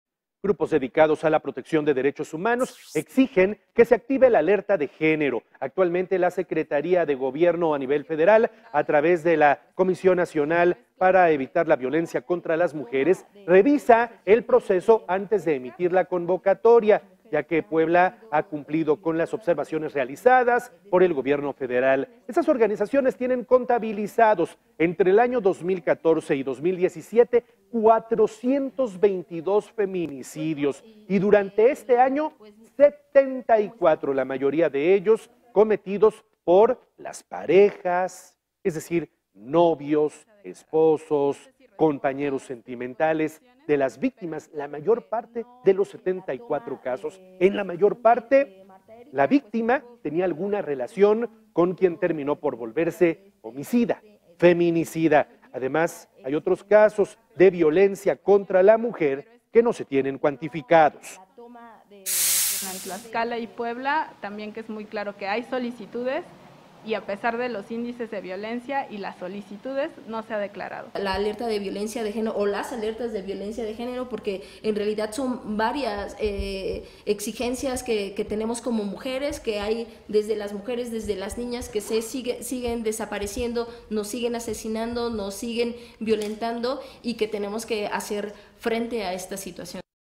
Piden alerta de género || Noticias con Juan Carlos Valerio
Grupos dedicados a la protección de derechos humanos exigen se active la alerta de género.